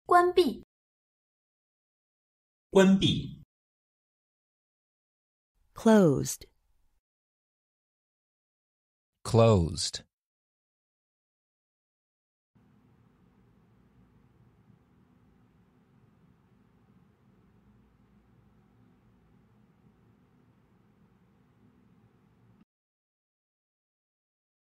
How to say "Closed" in Chinese (Simplified)
Male Voice:
关闭

Female Voice:
关闭

Male Voice Translation:
Closed

Female Voice Translation:
Closed